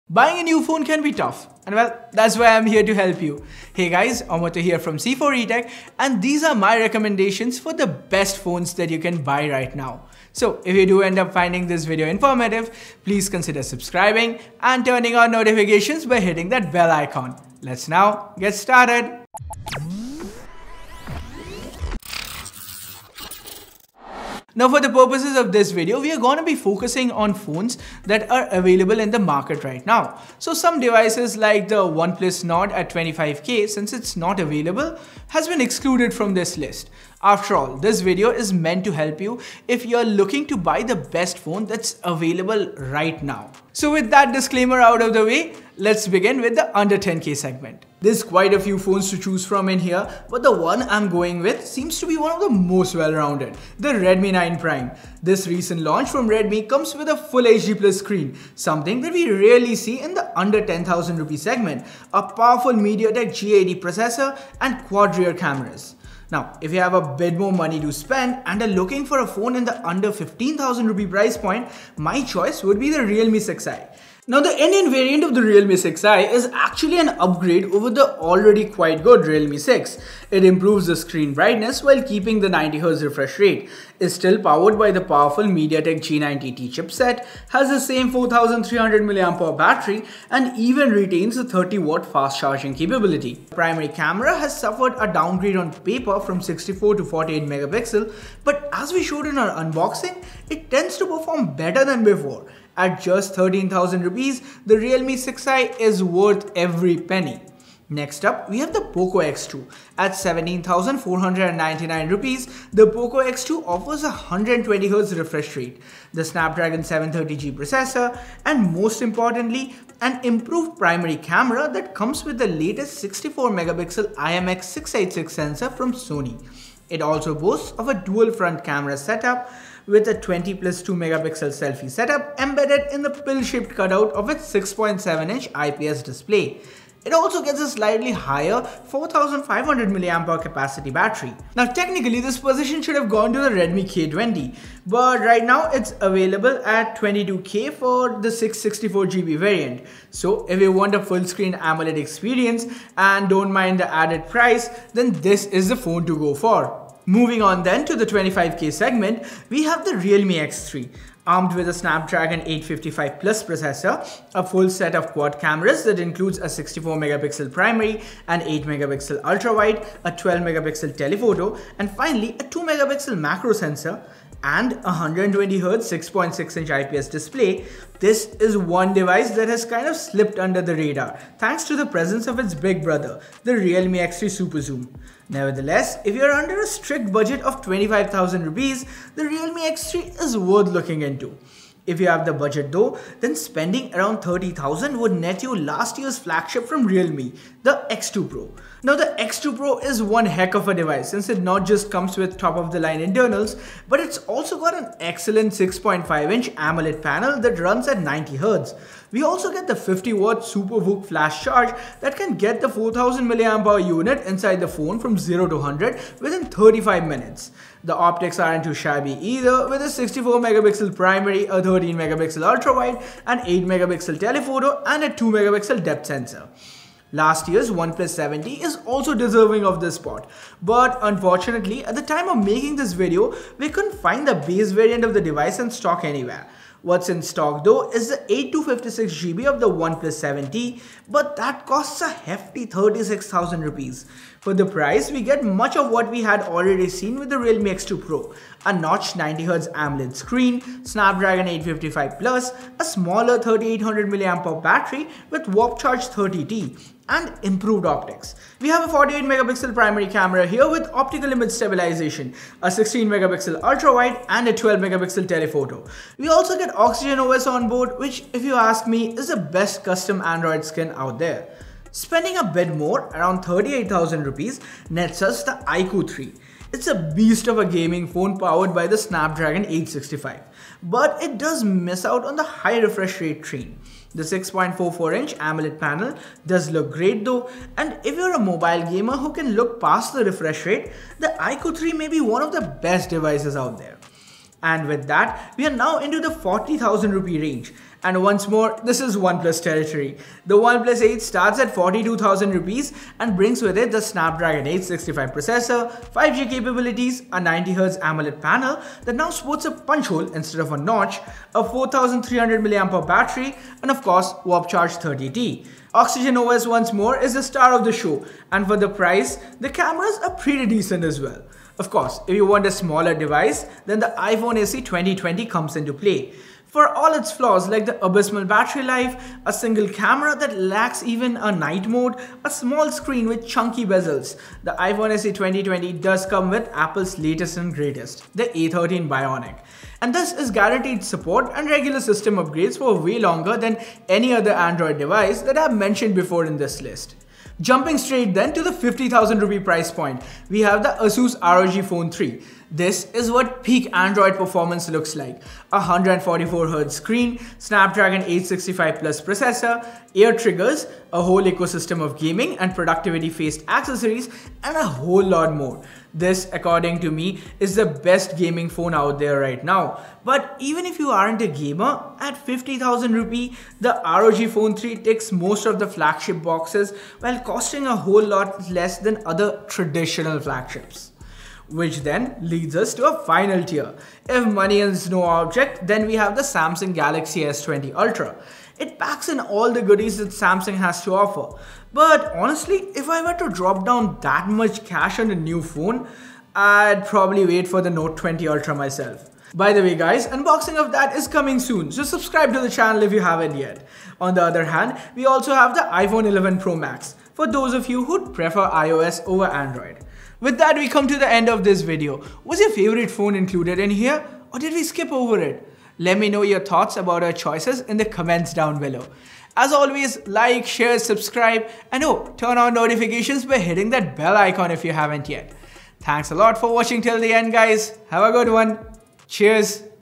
Best Phones to Buy at Every Price Point (August 2020)
In this video I take you through my picks for the best smartphone in each major price category.

We have a lot of choices nowadays with so many phones being launched so it can be kind of hard to choose a phone in a specific budget - we have made it easier for you guys by choosing the best smartphone possible at all the popular price points!